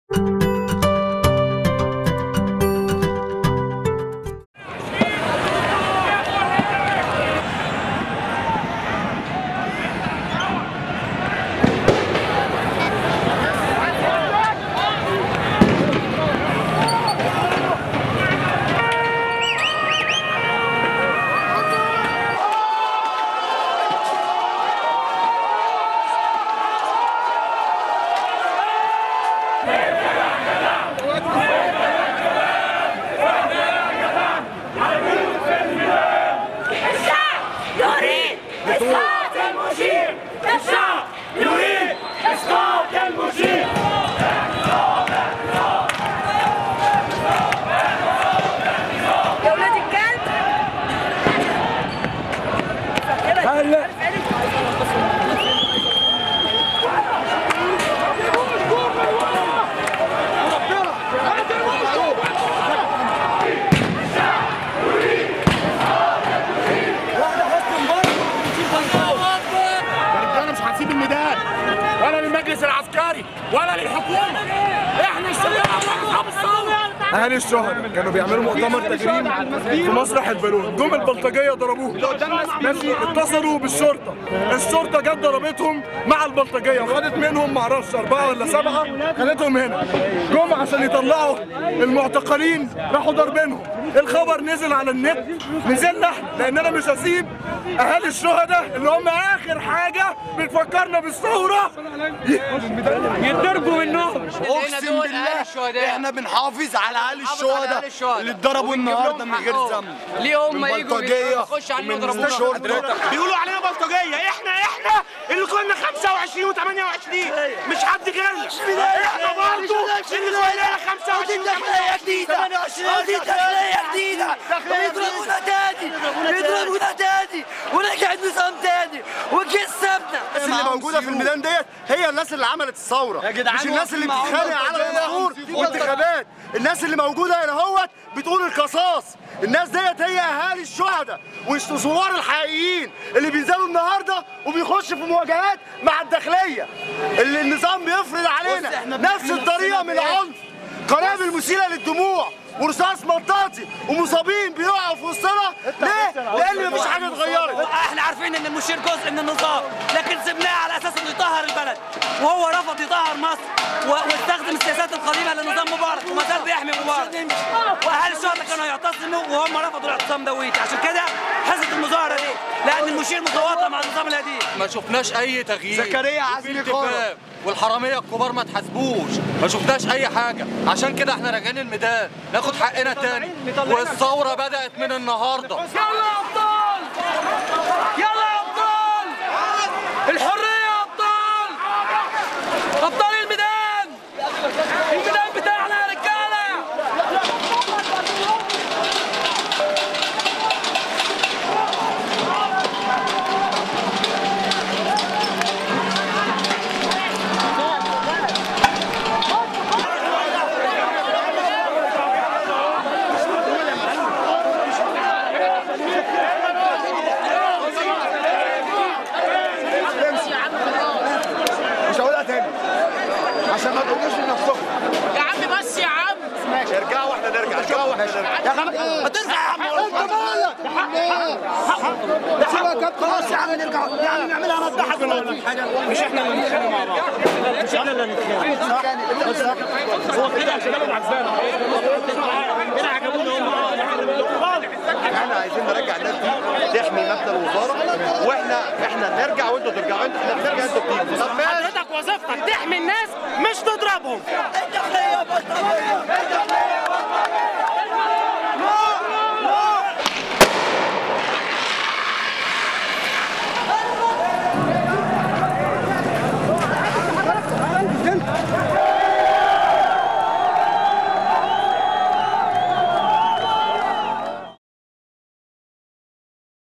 «المتظاهرون يعودون لـ«الميدان
تحول ميدان التحرير مرة أخرى إلى ساحة معركة، ووقعت اشتباكات بين الشرطة ومتظاهرين أثناء تجمع مئات من أسر شهداء  ثورة 25 يناير بميدان التحرير للمطالبة بسرعة محاكمة المسؤولين عن مقتل أبنائهم وأقاربهم، استخدمت الشرطة بكثافة القنابل المسيلة للدموع بينما ألقى المتظاهرون الزجاجات الحارقة والحجارة على قوات الشرطة. استمرت المصادمات طوال الليل، وعلى مدار اليوم التالي، وأسفرت عن إصابة 1114 شخصا، وفقا لتقديرات وزارة الصحة، تم تحويل 75 منهم إلى المستشفيات، وعلاج الباقين على يد فرق المسعفين فى مكان الحادث.